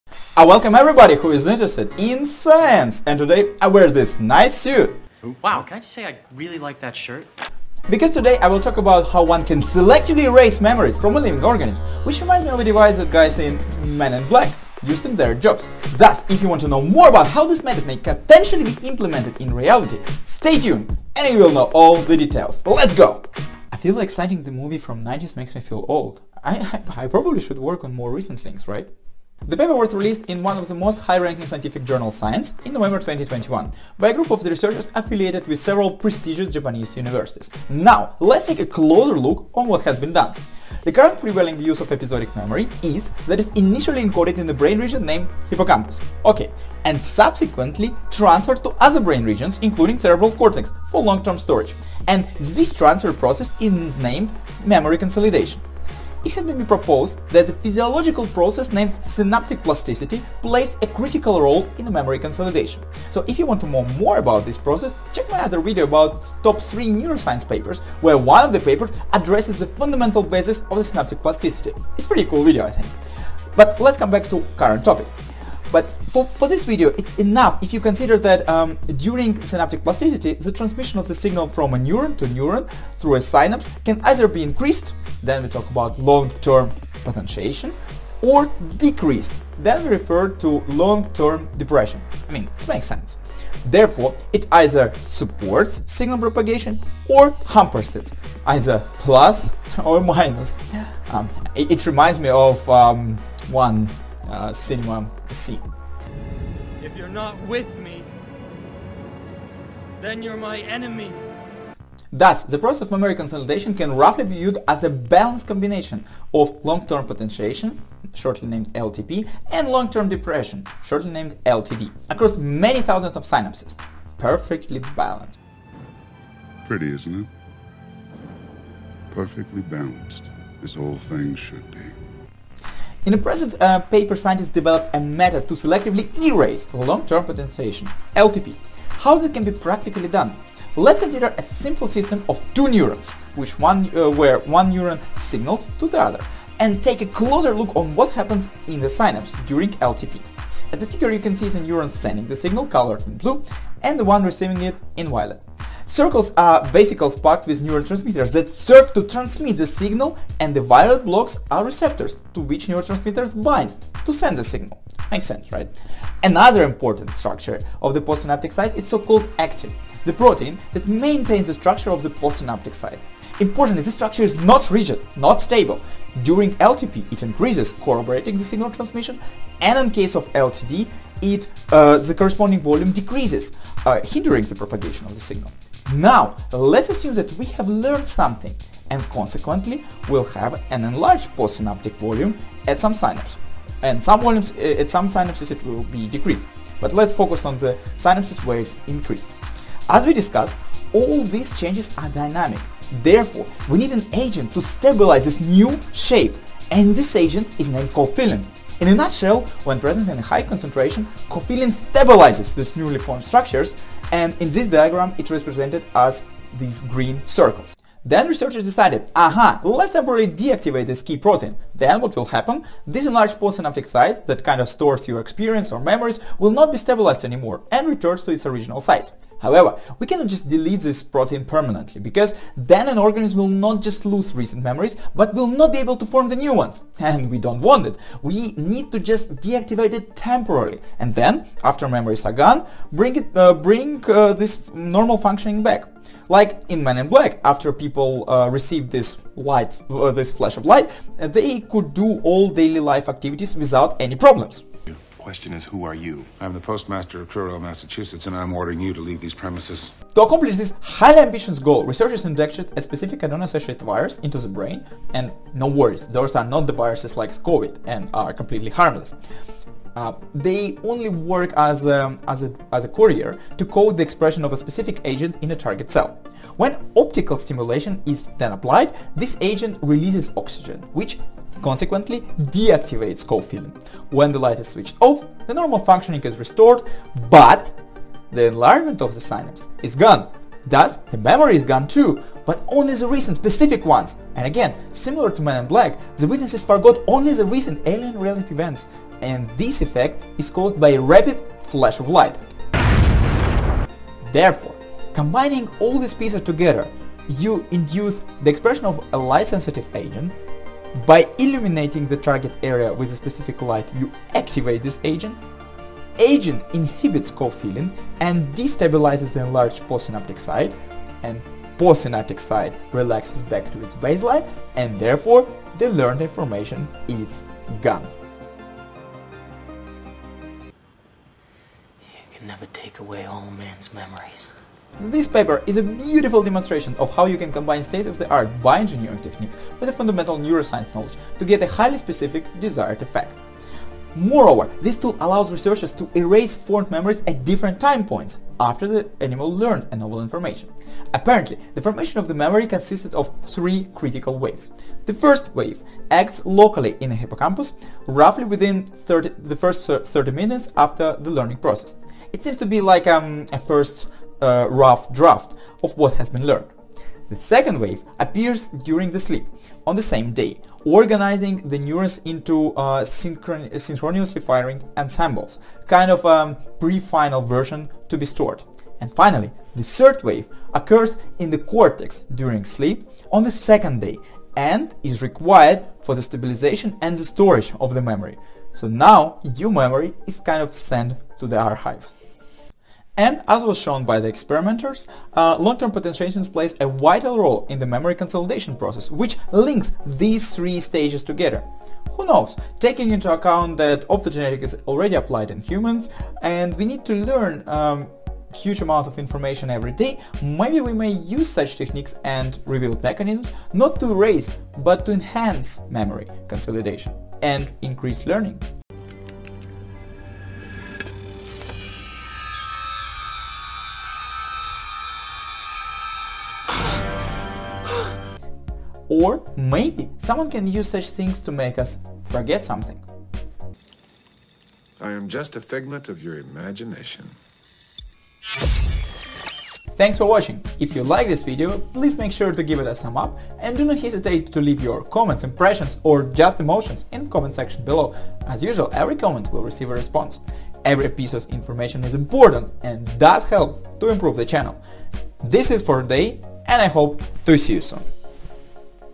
How did scientists erase memories (Eng subs)
Neuroscientists deleted recent episodic memories from mice by optogenetically destroying long-term potentiation (LTP).

Timecodes:
0:00-Introduction
0:29-Stepwise synaptic plasticity events drive the early phase of memory consolidation
0:42-Episodic memory and memory consolidation
0:57-Synaptic plasticity
1:23-Long Term Potentiation (LTP) and Long-Term-Depression (LTD)
2:24- LTP and LTD in the synapse.
3:28- Cofilin. Protein stabilizes actin.
3:38- Distablization of cofilin. Its effect on LTP and memories
4:21-Optogenetic approach to deactivate cofilin
5:44- Three waves of the memory consolidation. The role of LTP.
7:20- Summary

In this video I review a neuroscience paper named "Stepwise synaptic plasticity events drive the early phase of memory consolidation" published in scientific journal Science in November 2021.

Scientists at Kyoto University used a ioengineering technique named optogenetics - optical stimulation of the brain - to break down the synaptic protein cofilin. Cofilin stablizes F-actin at spine heads and, therefore, cofilin is an essential synaptic component required an for optimal synapse function. To perform the optogenetic deactivation of synaptic protein cofilin, an adenovirus is injected into the mice brain. Adeno-associated virus works as a "courier" to deliver a protein bound to a fluorescent marker. When optical stimulation is applied, the proteins release oxygen (e.g. reactive oxygen species, ROS), which deactivates cofillin. Due to this, it is possible to act locally on specific parts of the brain at a given point in time during the period of memory consolidation.

By using optogenetics to selectively block long-term potentiation (LTP) within a certain time period, neuroscientists have been able to find that different phases of synaptic plasticity play different roles during memory consolidation. The first wave occurs locally in the hippocampus. The second wave, which occurs during sleep on the same day, organizes neurons into synchronously activated ensembles. And the third wave occurs already in the anterior cingulate cortex of the brain during sleep on the second day, which is necessary for further stabilization of memory.